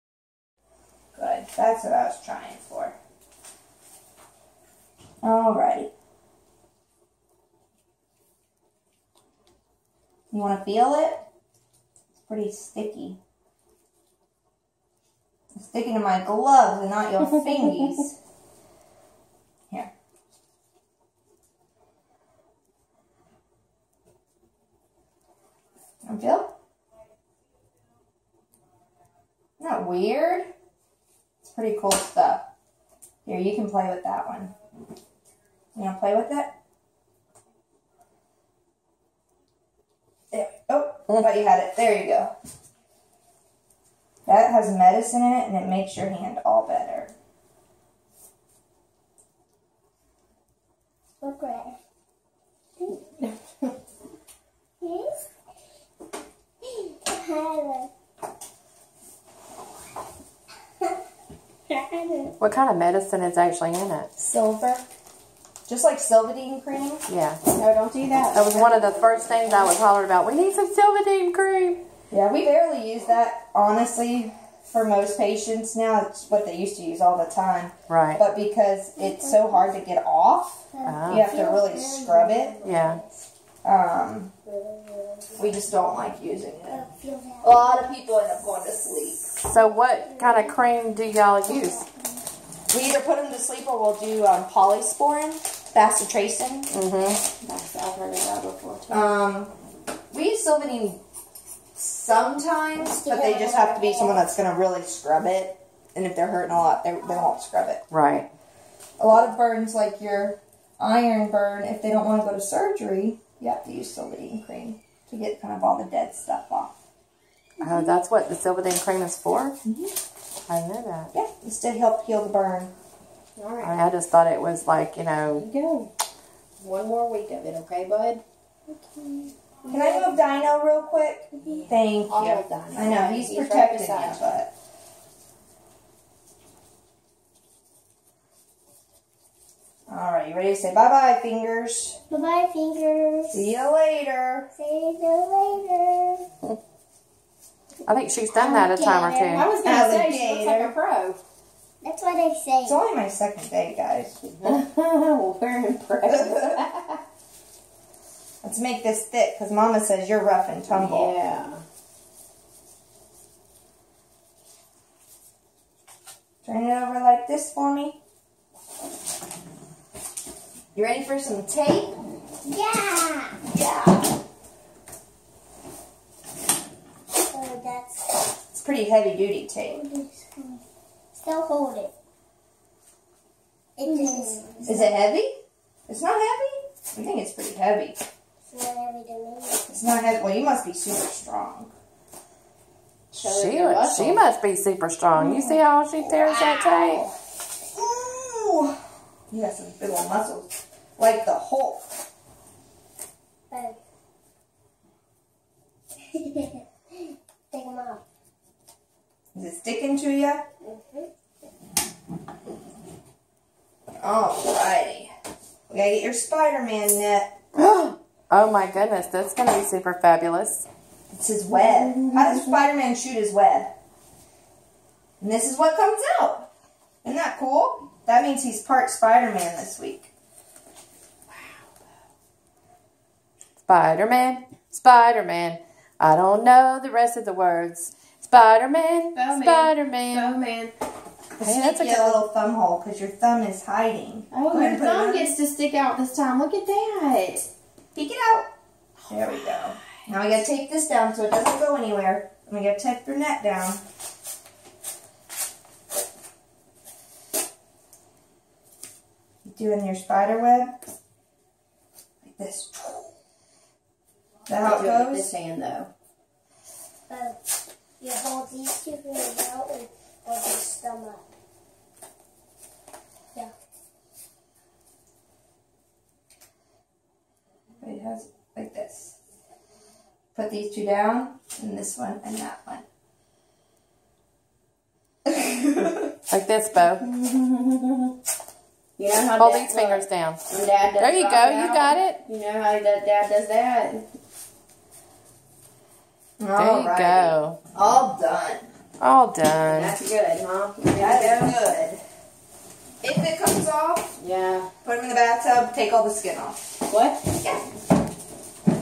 Baby Bo Burn check up part 2
3 year old Baby Bo - his chair collapsed and he fell into the fire pit burning his hand  2nd degree burns.
Flew in helicopter from South Carolina to the burn unit in Augusta Ga